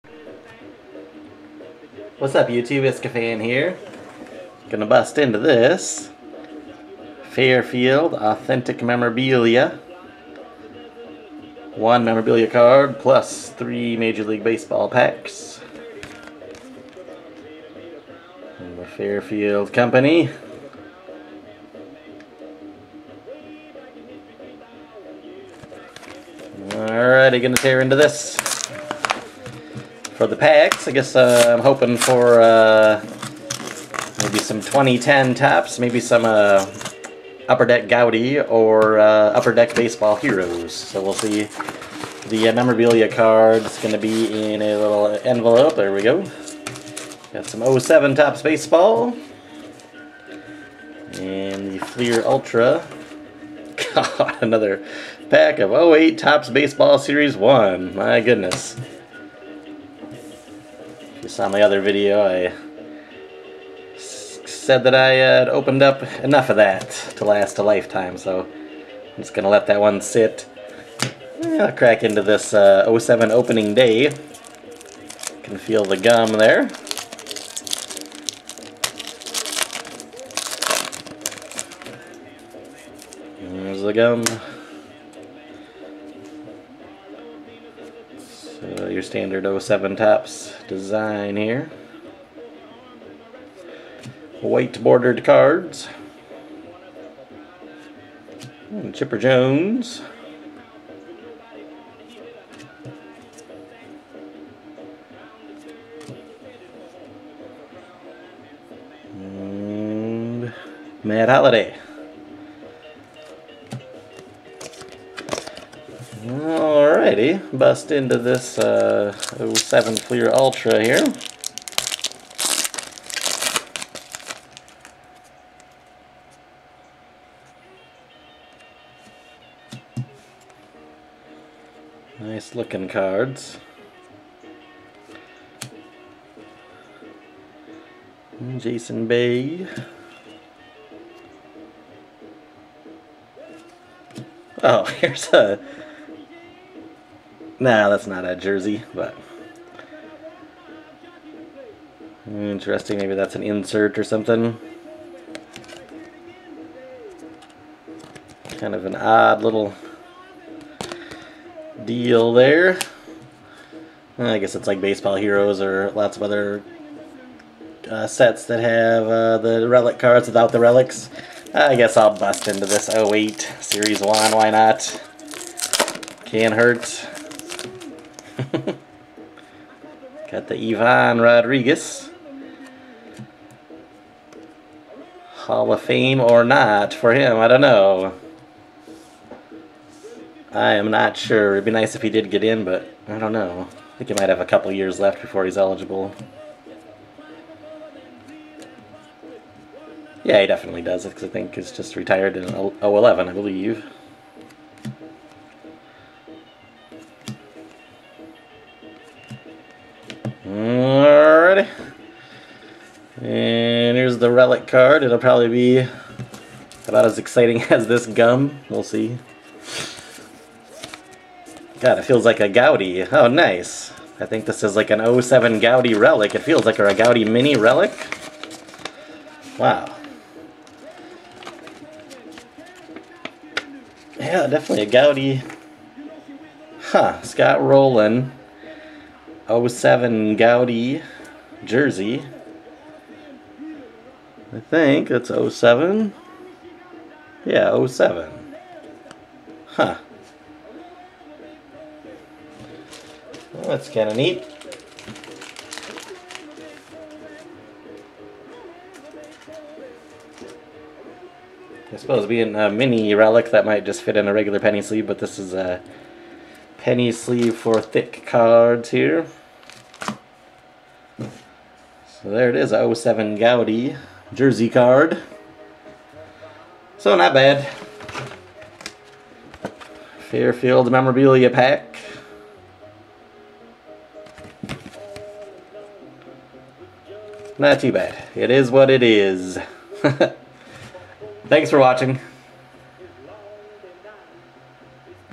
Authentic Memorabilia Fairfield Break
Three packs and one very solid relic card.